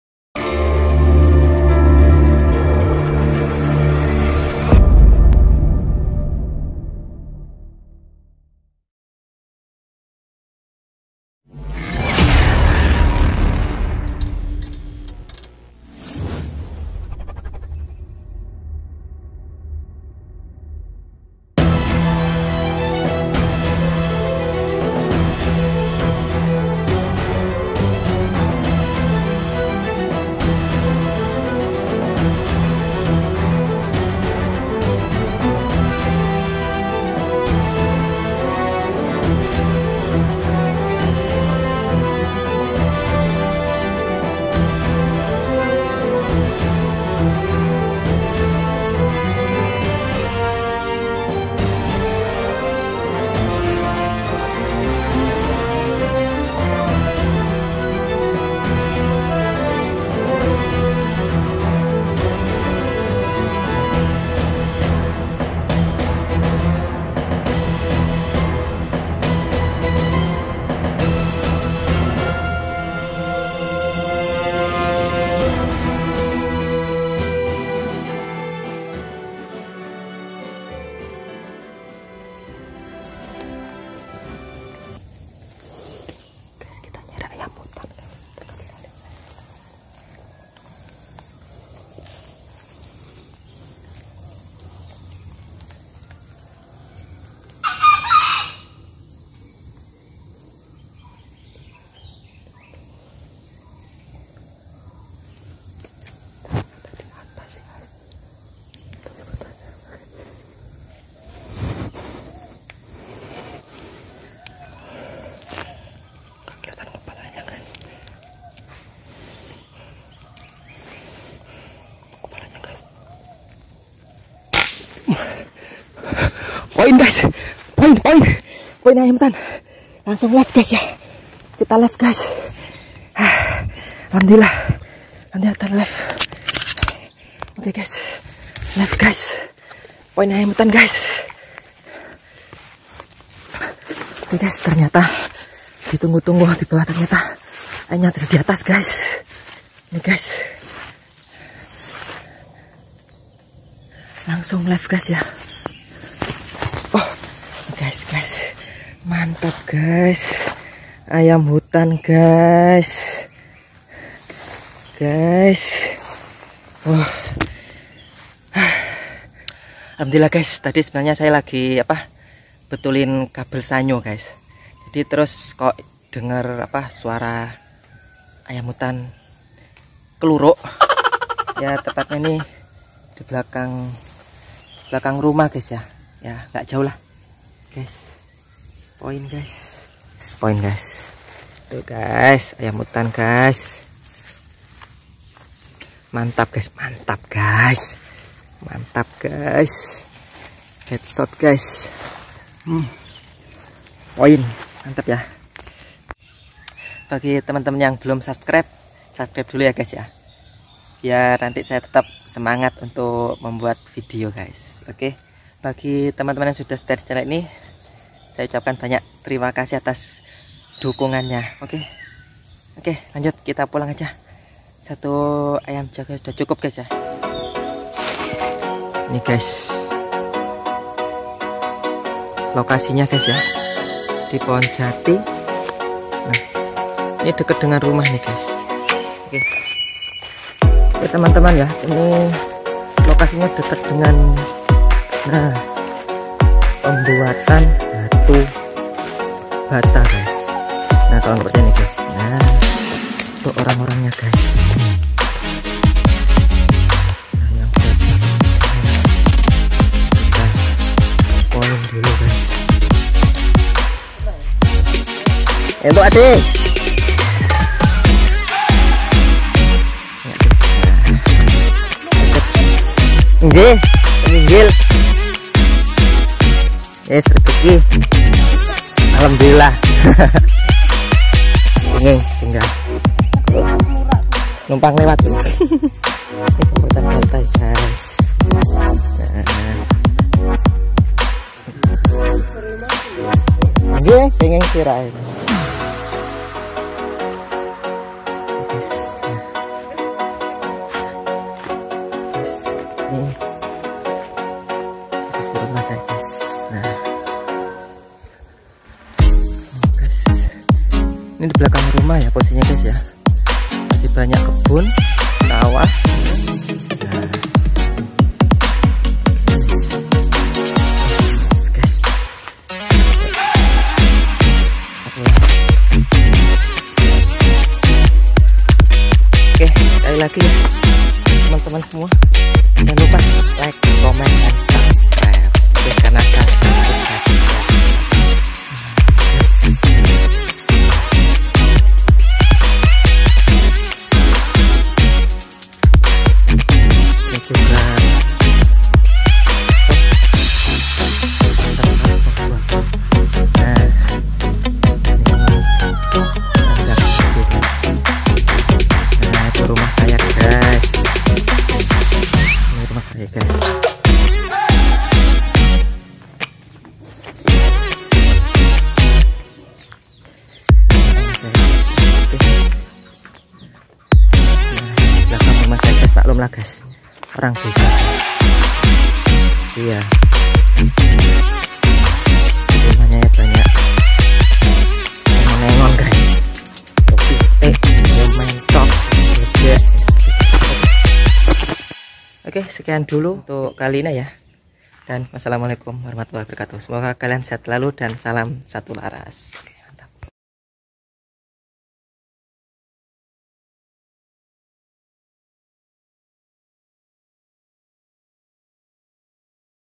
Berburu Ayam Hutan || Poin Yang Tak Terduga
Sehabis membetulkan kabel sanyo yang putus,saya mendengar suara kokok ayam hutan.kemudian saya coba cek,ternyata saya mendapatkan poin tak terduga,ayam hutan ada diatas saya.
Mau tau keseruannya,tonton terus videonya ya gaessss.